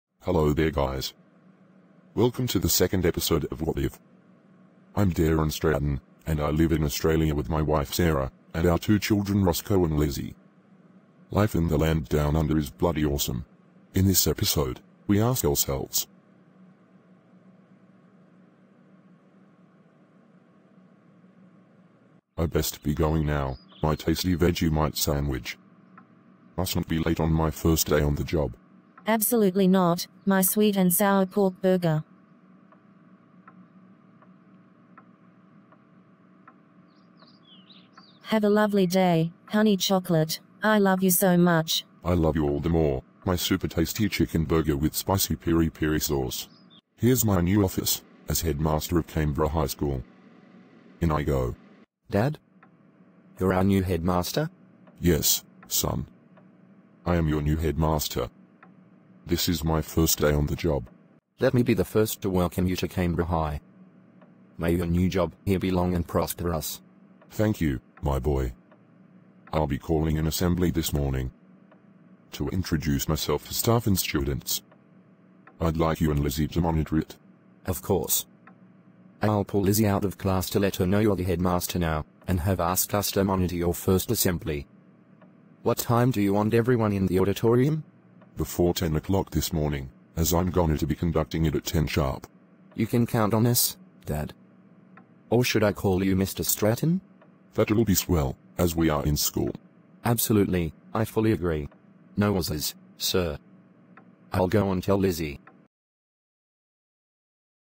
01:002: What If All of The Stratton Family Were Australian?
What If: Series 1, Episode 2